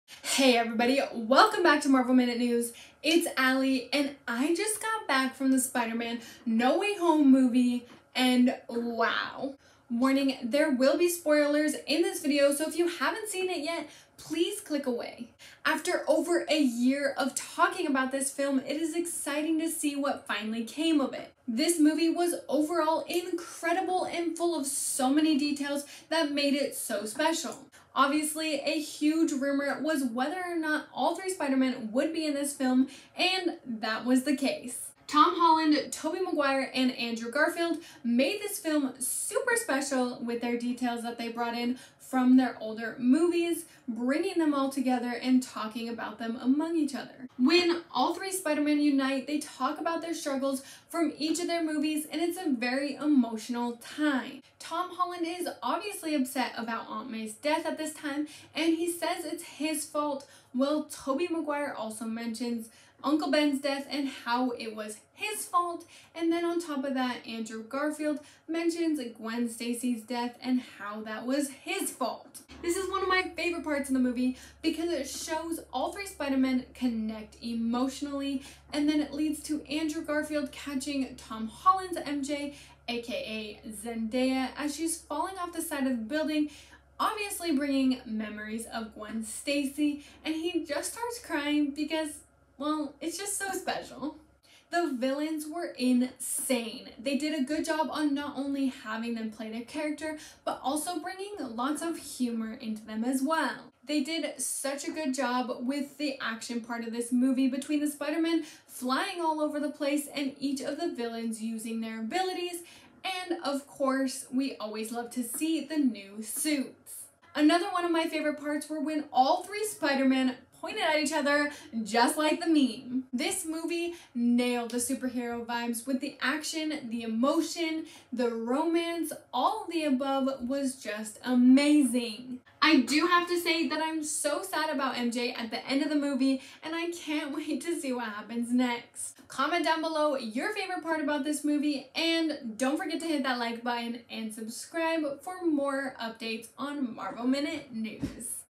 I Just Saw Spider-Man No Way Home MY REACTION! (Spoilers Ahead)
Here are my thoughts of the long awaited Spider-Man no way home!

Welcome to marvel minute news where we fast track your marvel updates & cut through all the slow intros and babbler.